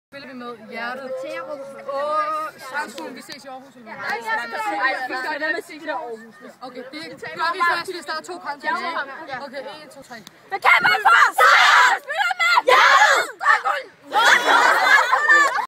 Kampråb fra Strandskolens piger
Glade piger med stor sejrsvilje før kampen mod Karlslunde Skole